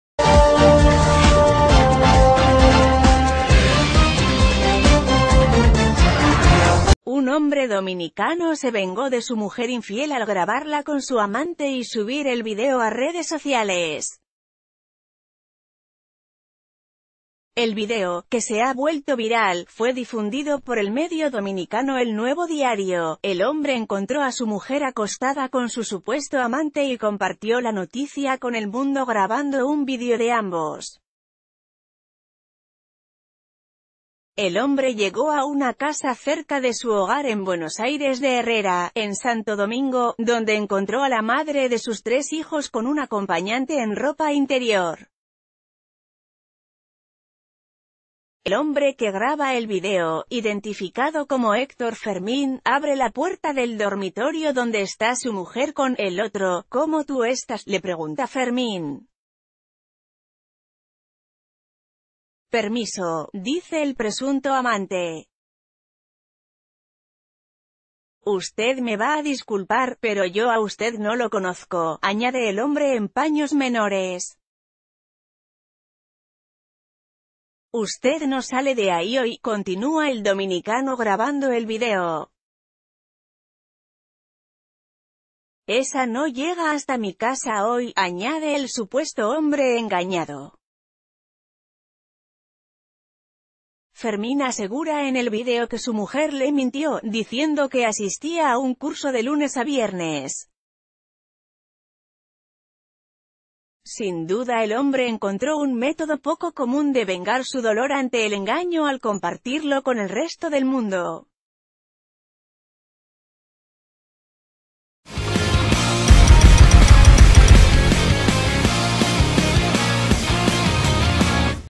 Dominicano se venga de pareja infiel grabándola con su supuesto amante y subiendo video a redes s...
El video, que se ha vuelto viral, fue difundido por el medio dominicano El nuevo diario. El hombre encontró a su mujer acostada con su supuesto amante y compartió la noticia con el mundo gra...